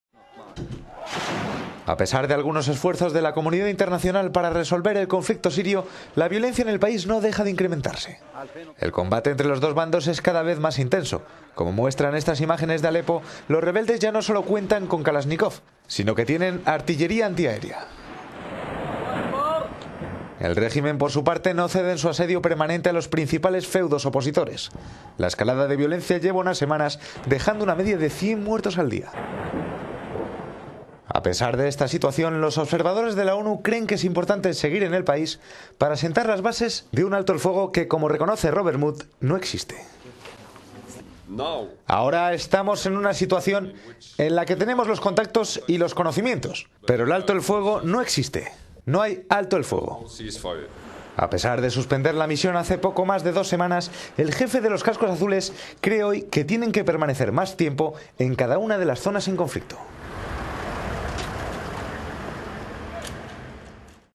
El aumento de la violencia enquista el proceso de paz en Siria
A pesar de algunos esfuerzos de la comunidad internacional para resolver el conflicto sirio, la violencia en el país no deja de incrementarse.

El combate entre los dos bandos es cada vez más intenso. Los rebeldes ya no solo cuentan con Kalachnikovs sino que tienen artillería antiaérea.

El régimen, por su parte, no cede en su asedio permanente a los principales feudos opositores.

La escalada de violencia lleva unas semanas semanas dejando una media de 100 muertos al día.

"Ahora estamos en una situación en la que tenemos los contactos y los conocimientos pero el alto el fuego no existe, no hay alto el fuego", declaraba el jefe de los cascos azules desplegados en Siria, Robert Mood.

A pesar de que el fin de la violencia parece estar lejos, los observadores de la ONU creen que es importante seguir en el país.

Hace poco más de dos semanas el jefe de los cascos azules dijo que suspendía la misión por la imposibilidad de hacer su trabajo. Hoy considera que los observadores tienen que permanecer más tiempo en las zonas en conflicto para examinar con detenimiento la situación.